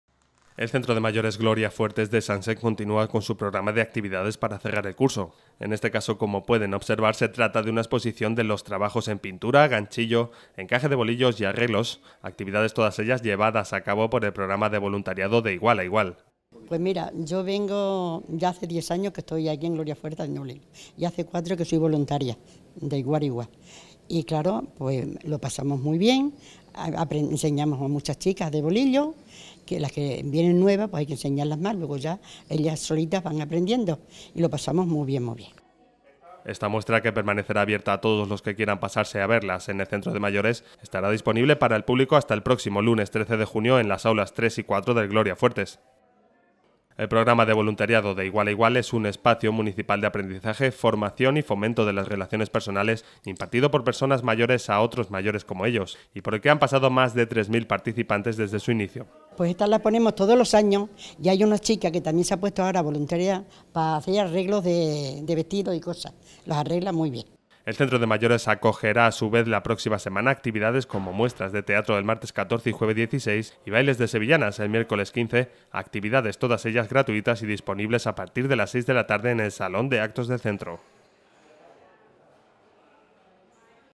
El Centro de Mayores expone los trabajos del programa De igual a igual
David CAZALLAS / San Sebastián de los Reyes, Madrid / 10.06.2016
El Centro de Mayores Gloria Fuertes continúa con su programa de actividades para cerrar el curso. En este caso se trata de una exposición de los trabajos en pintura, ganchillo, encaje de bolillos y arreglos, actividades todas ellas llevadas a cabo por el programa de voluntariado De igual a igual.

Esta muestra, que permanecerá abierta a todos los que quieran pasarse a verlas en el Centro de Mayores, estará disponible para el público hasta el próximo lunes 13 de junio en las Aulas 3 y 4 del Gloria Fuertes.

El Centro de Mayores acogerá a su vez la próxima semana actividades como Muestras de Teatro el martes 14 y jueves 16 y bailes de Sevillanas, el miércoles 15, actividades todas ellas gratuitas y disponibles a partir de las seis de la tarde en el Salón de Actos del Centro.